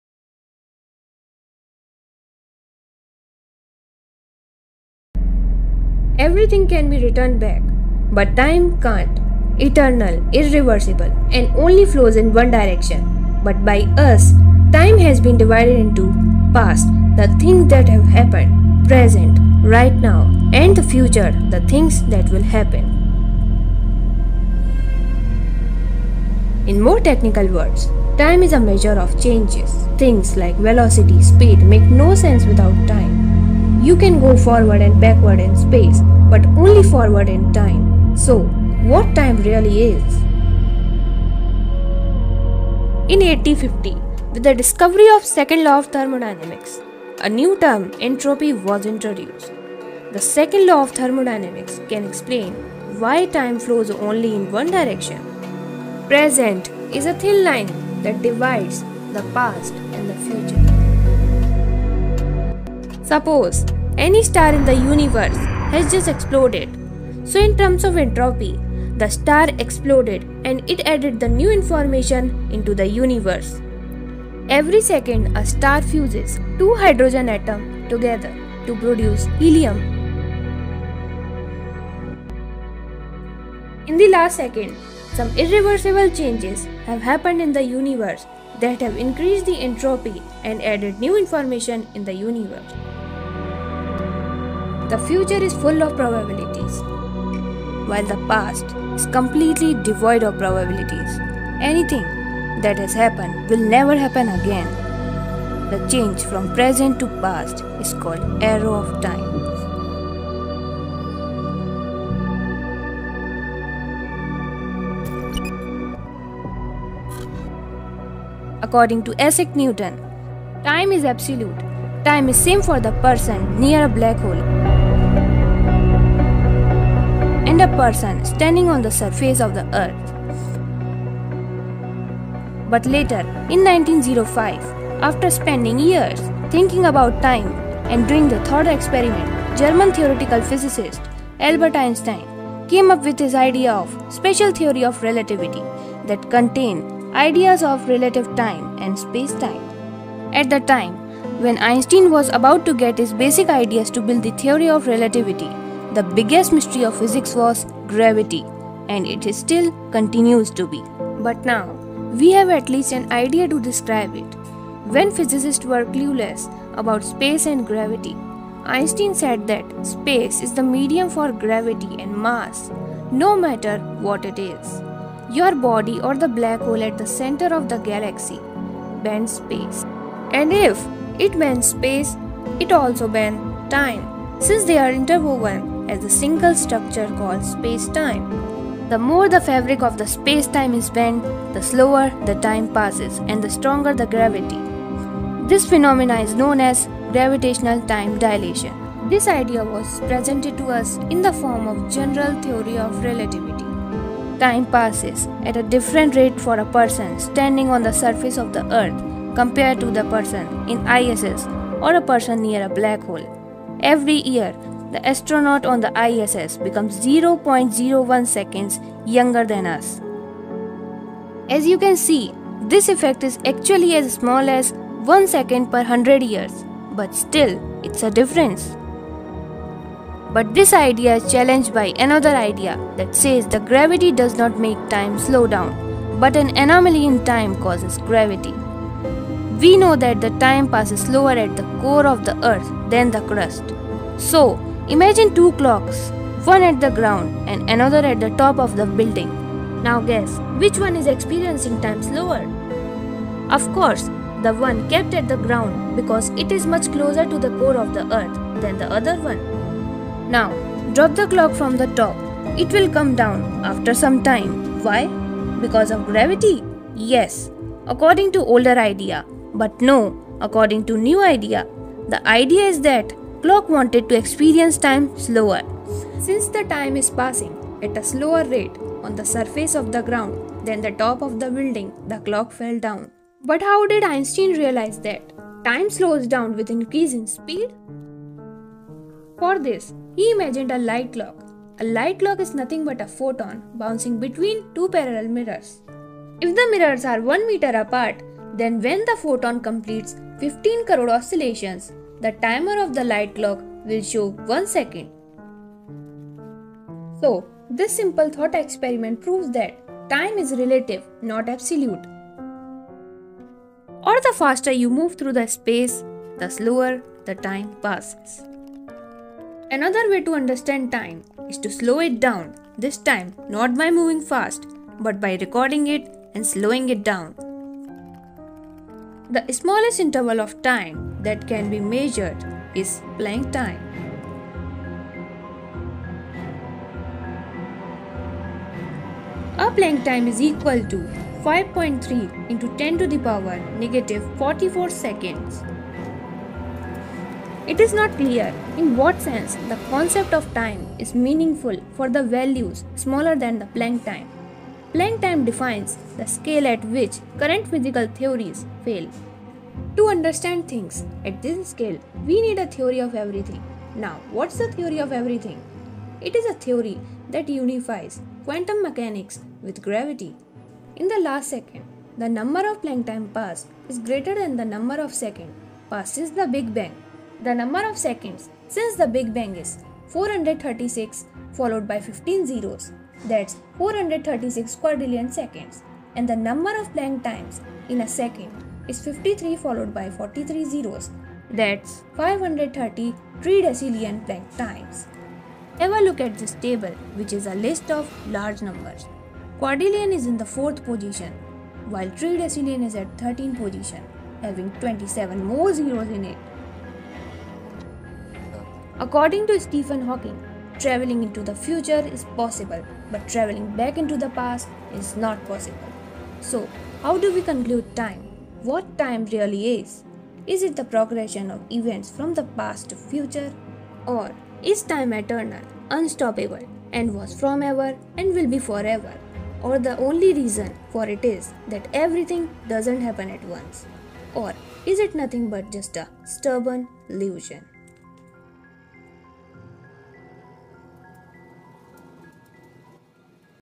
Time in a nutshell ⏰
What time really is?
Is time eternal, unstoppable?
Is time fromever and will be forever?
Or it is nothing but stubborn illusion?
Time is always be a mystery for everyone to slove that out we have many theories which have been discusses in this video.

Chapters-
1. Introduction 0:00
2. Entropy 0:43
3. Arrow of time 1:20
4. Einstein's idea 1:58
5. Time's anomaly 3:24
6. Time slow down with speed 4:43
7. Planck time 5:20
8.  Conclusion 6:46